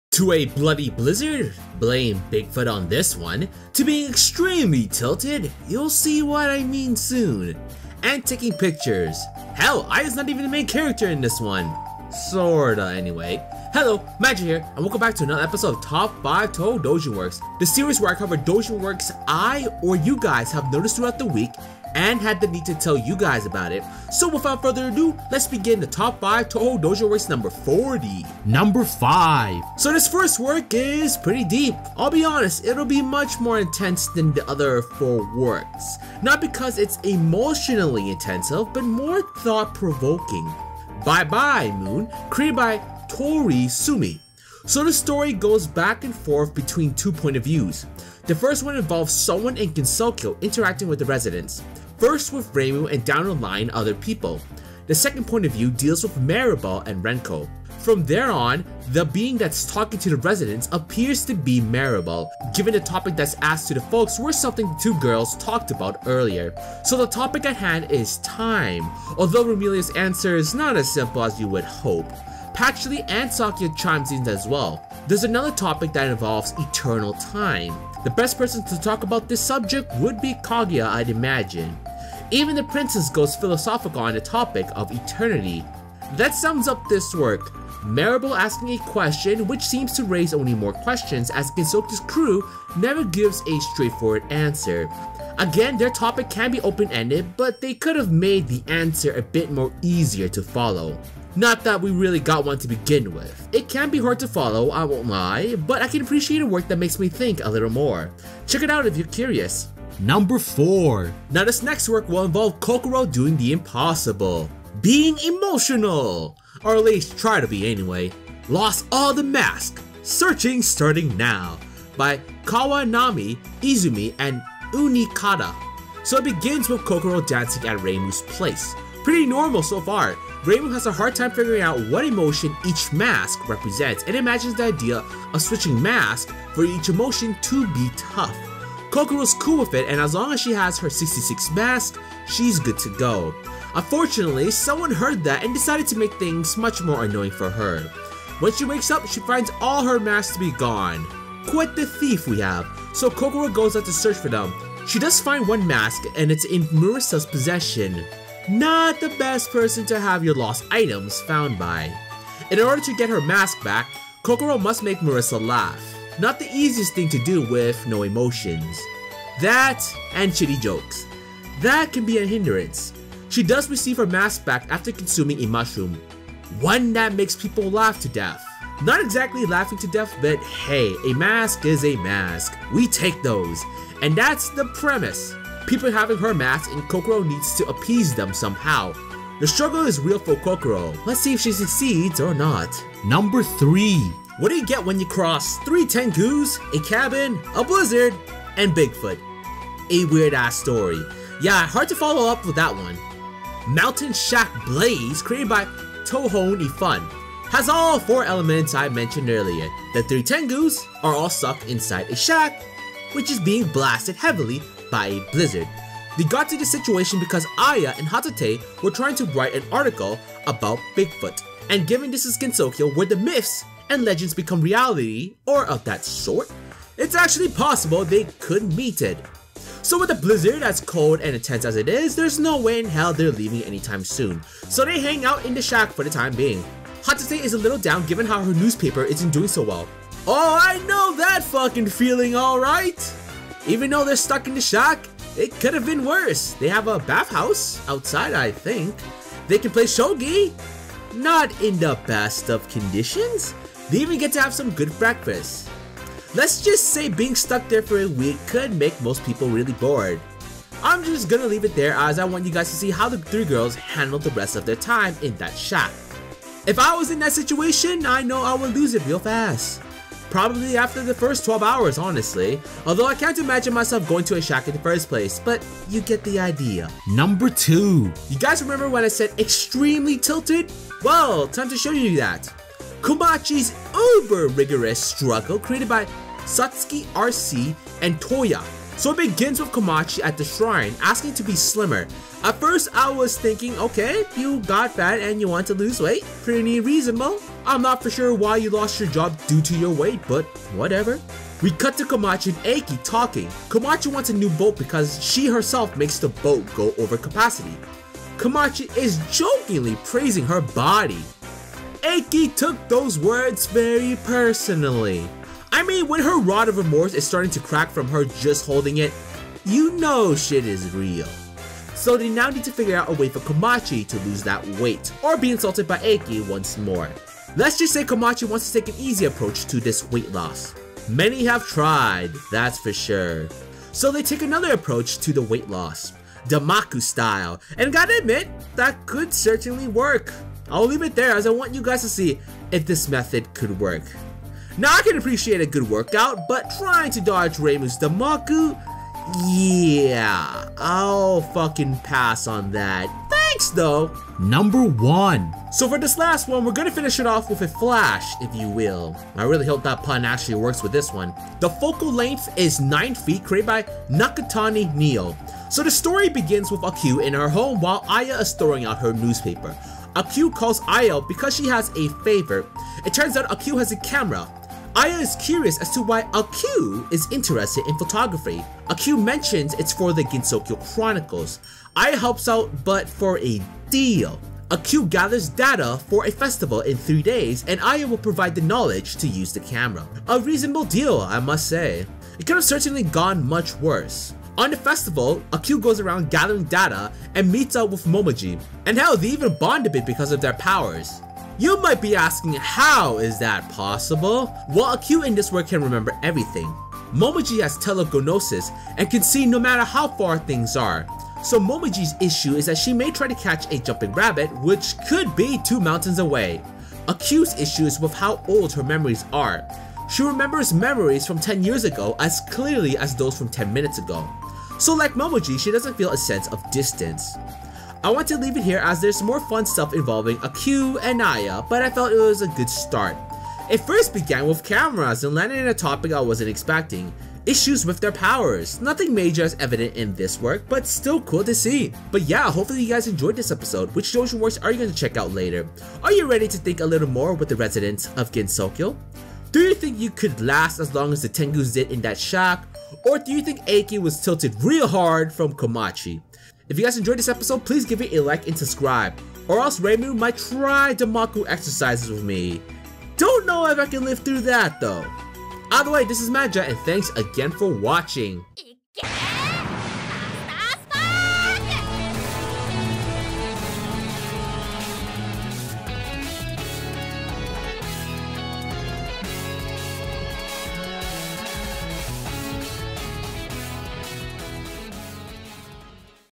Top 5 Touhou Doujin Works #40 (Bloody Blizzards!)
To a bloody blizzard. Blame bigfoot on this one. To being extremely tilted, you'll see what I mean soon.  And taking pictures. Hell, Aya's not even the main character in this one, sorta anyway. Hello, Magi here and welcome back to another episode of Top 5 Touhou Doujin Works.

Playlist for my Playthroughs.

v=3Gm8xGdooxE&list=PLJT0OtjwnENi8oWi_Q2RGBDOoI6z_i5VU

A New Series my friend and I are playing (Non Touhou related)